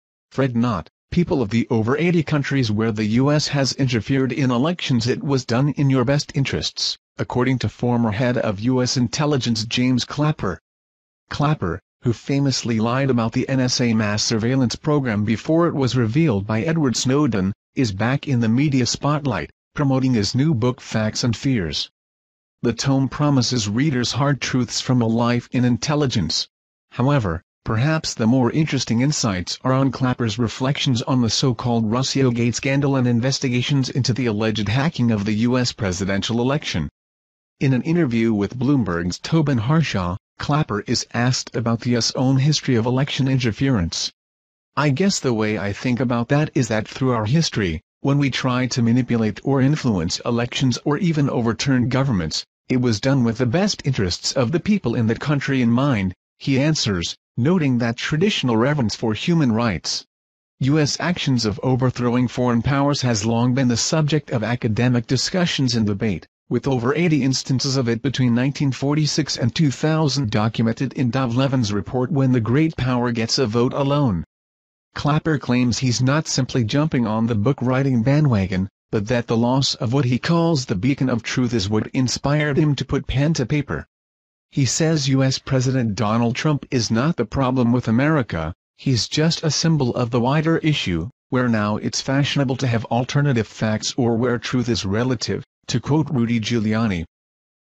The best interests of the people’: Ex-US top spy Clapper justifies election interference
Fret not, people of the over 80 countries where the US has interfered in elections – it was done in your “best interests,” according to former head of US Intelligence James Clapper.

Clapper, who famously lied about the NSA mass surveillance program before it was revealed by Edward Snowden, is back in the media spotlight, promoting his new book ‘Facts and Fears.’The tome promises readers ‘Hard truths from a life in intelligence.’ However, perhaps the more interesting insights are on Clapper’s reflections on the so-called Russiagate scandal and investigations into the alleged hacking of the US presidential election.

In an interview with Bloomberg’s Tobin Harshaw, Clapper is asked about the US’ own history of election interference. ‘I guess the way I think about that is that through our history, when we tried to manipulate or influence elections or even overturned governments, it was done with the best interests of the people in that country in mind,’ he answers, noting that “traditio